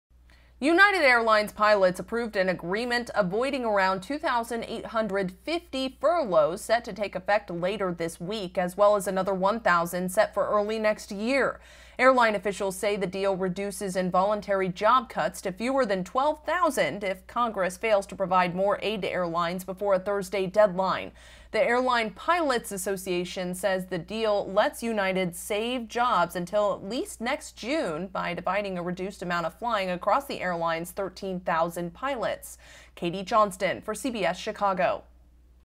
United Airlines Pilots Approve Deal To Avoid Furloughs Until June
Pilots at United Airlines voted to approve an agreement that will avoid about 2,850 furloughs that were set to take effect later this week and another 1,000 early next year. Katie Johnston reports.